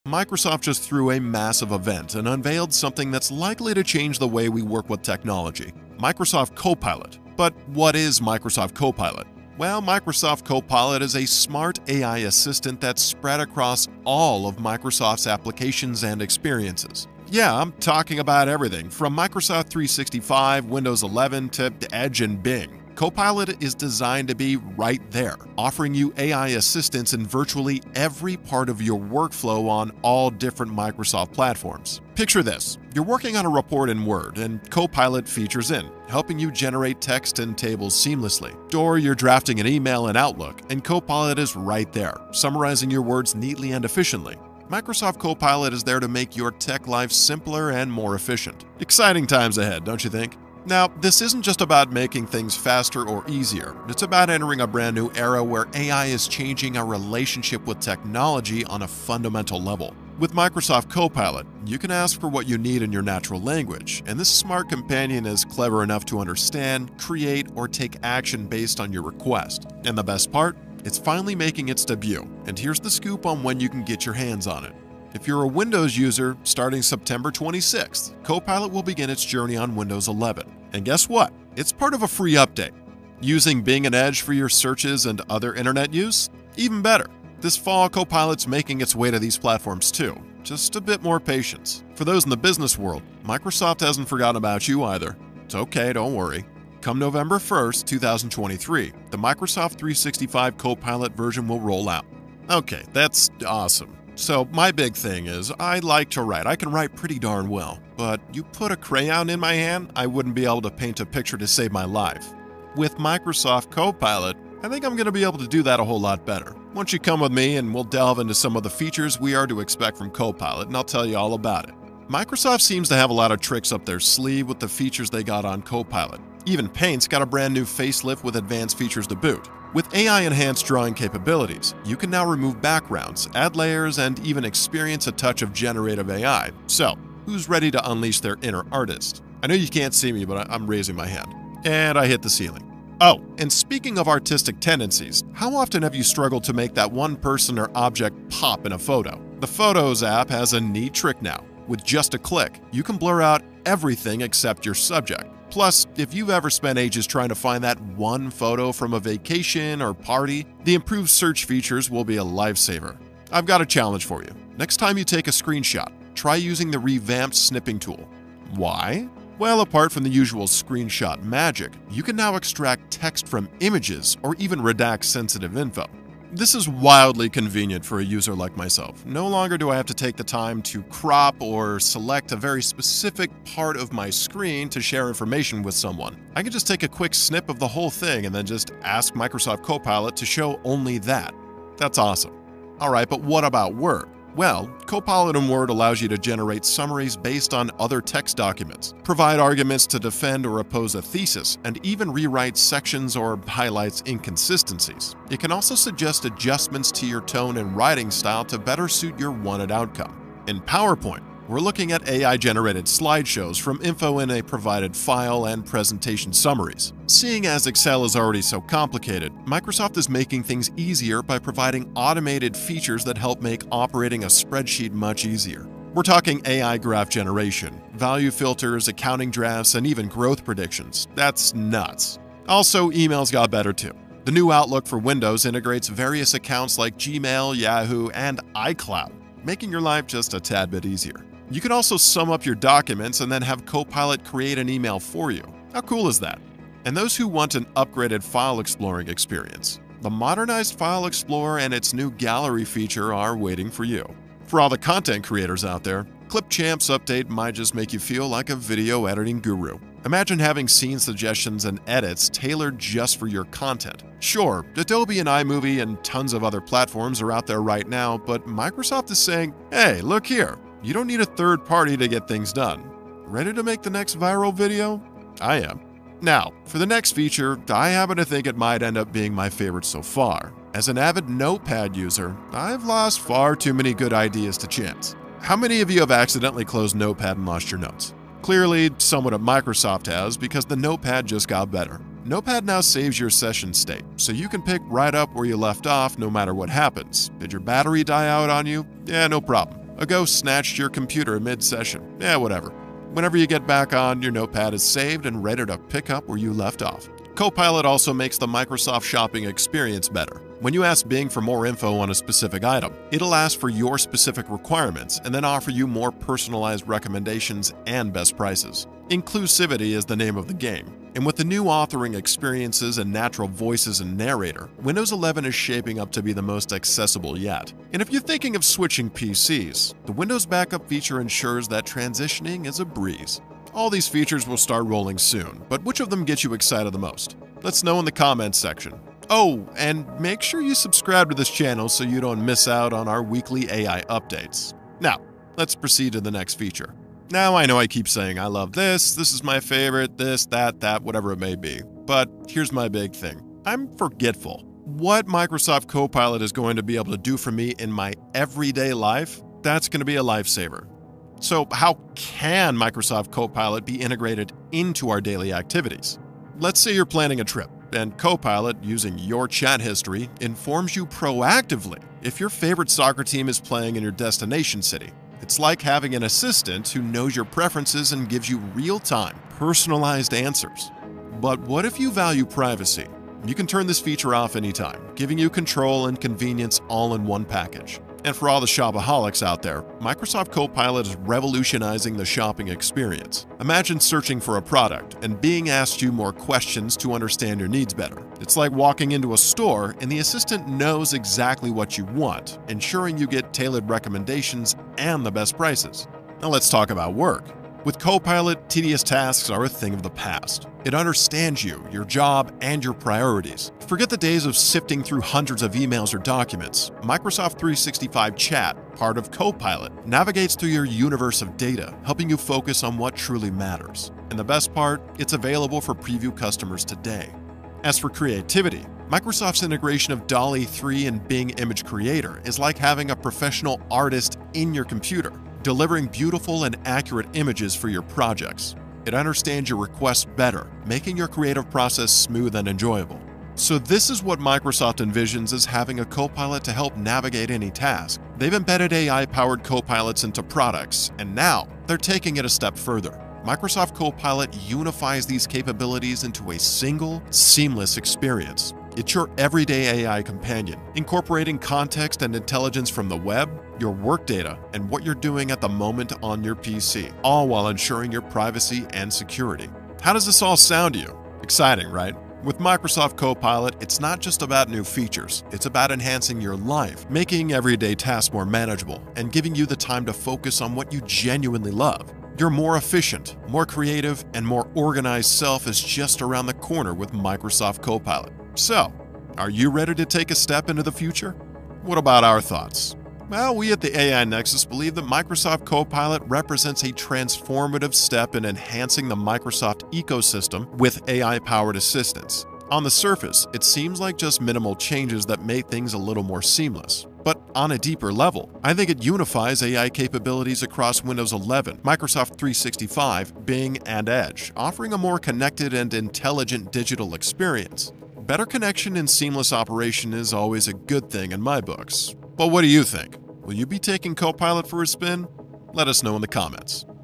Microsoft COPILOT Will Blow Your MIND!
Get ready for a coding revolution with Microsoft Copilot! This mind-blowing tool redefines the way you code, offering unprecedented features and innovation that will elevate your programming experience to new heights. Embrace the future of coding with Copilot!